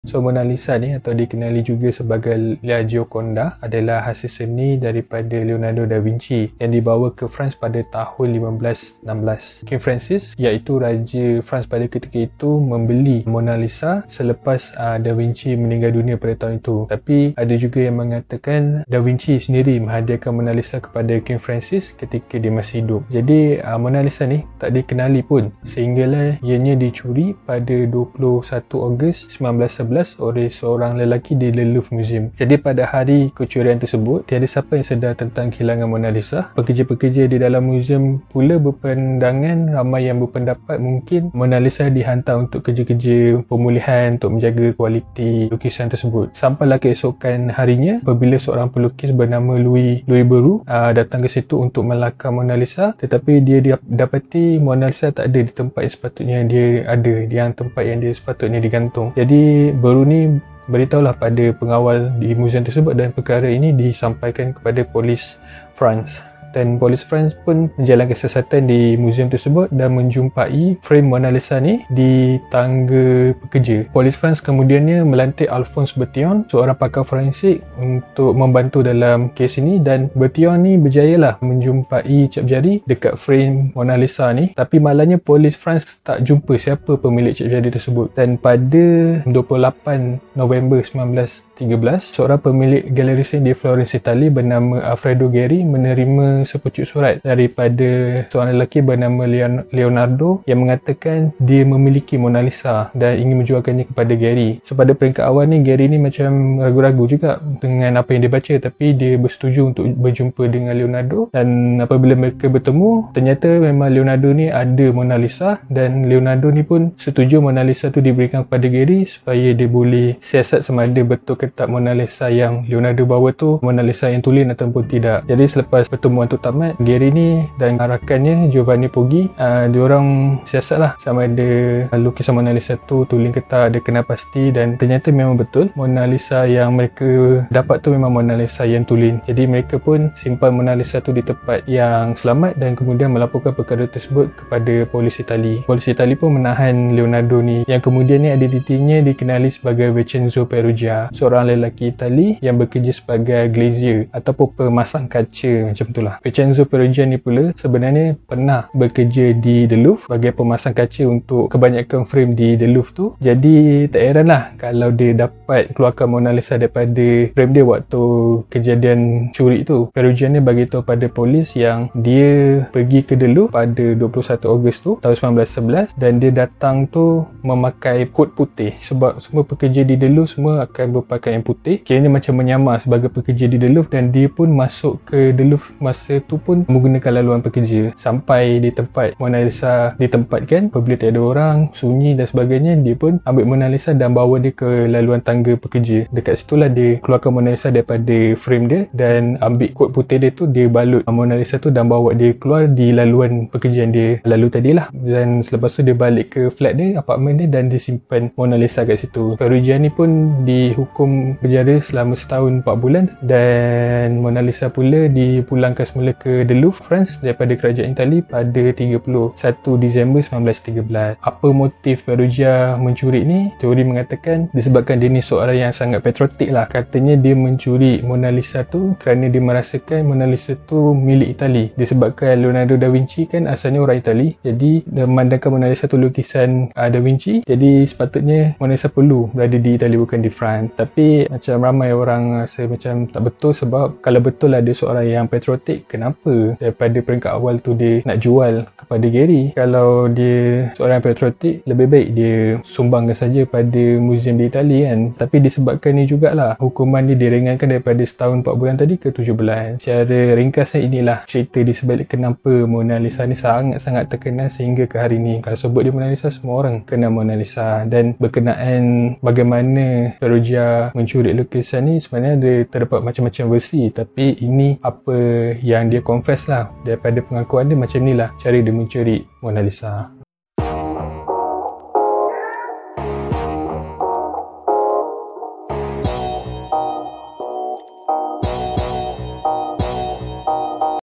Peristiwa Kecurian Lukisan Mona Lisa
Peristiwa yang telah mempopularkan lukisan ni. Sebelum tu tak berapa famous.